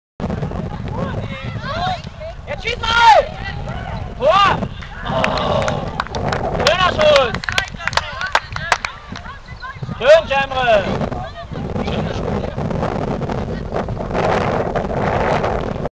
19.04.2009 C 1 TSV Gräfelfing Oly Moosach
Videos vom 19.04.09 unserer C1 Jungs beim Punkspiel gegen den TSV Gräfelfing